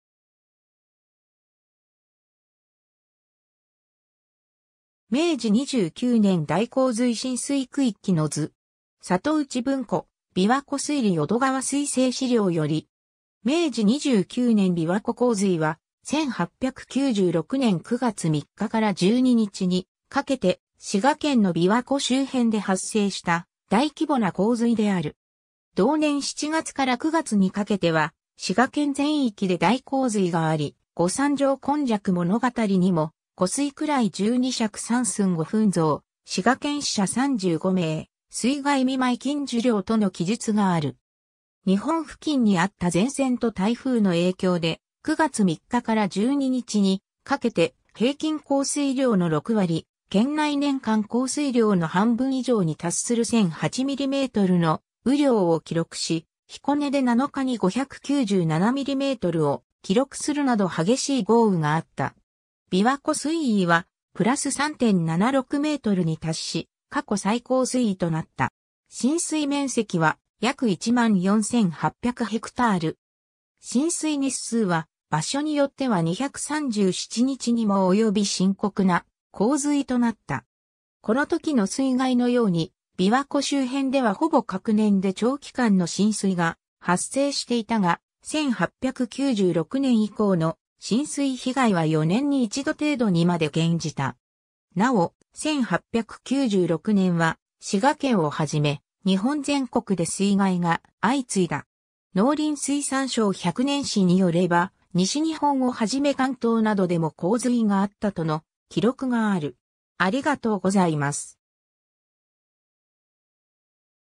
明治29年琵琶湖洪水
明治二十九年大洪水浸水区域之図。
『里内文庫』「琵琶湖水理淀川水制資料」（栗東歴史民俗博物館蔵）より。
 明治29年琵琶湖洪水（めいじ29ねんびわここうずい）は、1896年（明治29年）9月3日から12日にかけて、滋賀県の琵琶湖周辺で発生した大規模な洪水である。
同年7月から9月にかけては、滋賀県全域で大洪水があり、『後三條今昔ものがたり』にも「湖水位12尺3寸5分増・滋賀県死者35名・水害見舞金受領」との記述がある。
 日本付近にあった前線と台風の影響で、9月3日から12日にかけて、平均降水量の6割・県内年間降水量（約1,900ミリメートル）の半分以上に達する1008ミリメートルの雨量を記録し、彦根で7日に597ミリメートルを記録するなど激しい豪雨があった。
琵琶湖水位は、プラス3.76メートルに達し、過去最高水位となった。
浸水面積は約14800ヘクタール。
浸水日数は、場所によっては237日にも及び深刻な洪水となった。
この時の水害のように、琵琶湖周辺ではほぼ隔年で長期間の浸水が発生していたが、1896年以降の浸水被害は4年に1度程度にまで減じた。
なお、1896年は滋賀県をはじめ、日本全国で水害が相次いだ。
『農林水産省100年史』によれば、西日本をはじめ関東などでも洪水があったとの記録がある。